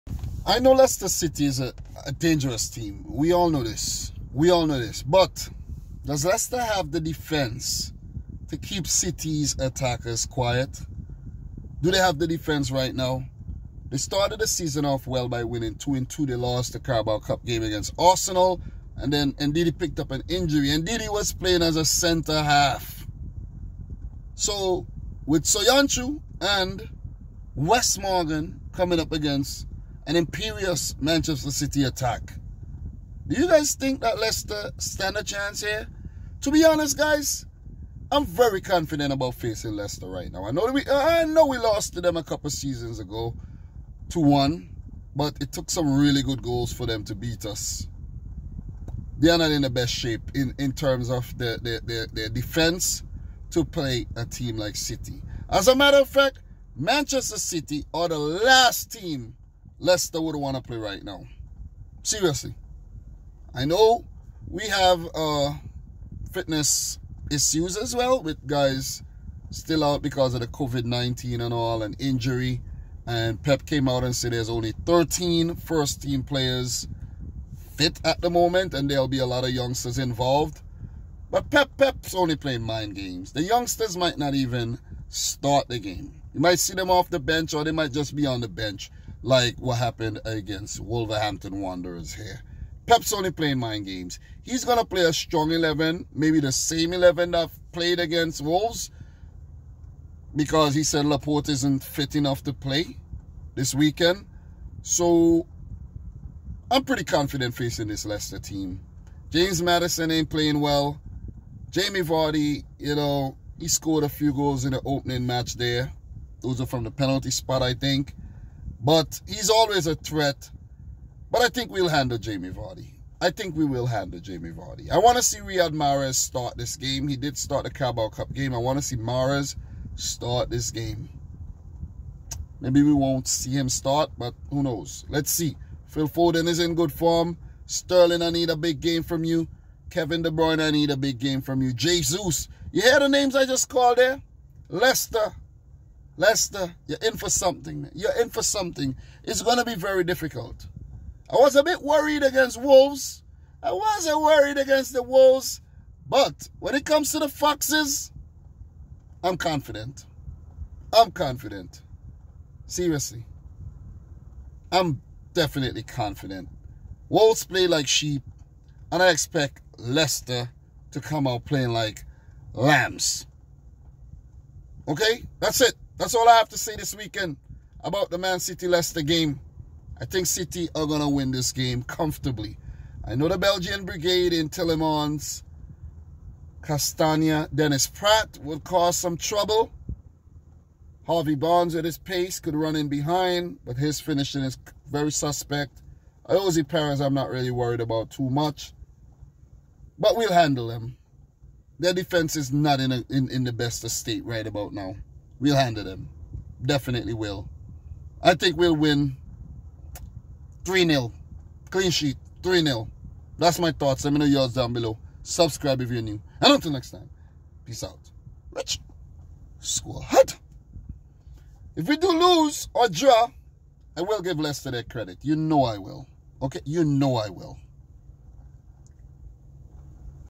Leicester Can Hurt Us But I'm Not Worried! Man City vs Leicester Preview
Dominic